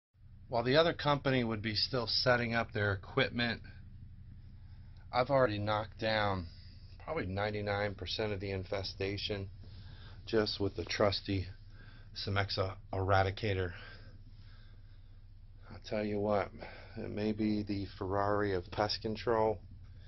Polti Cimex Eradicator in action - Video by Pest Peppers
- Polti Cimex Eradicator is a steam generator with a patented device that superheats the steam to 180°C. 💨
- The steam eliminates bedbugs and their eggs through thermal shock right from the first pass, as these parasites are extremely sensitive to temperature changes.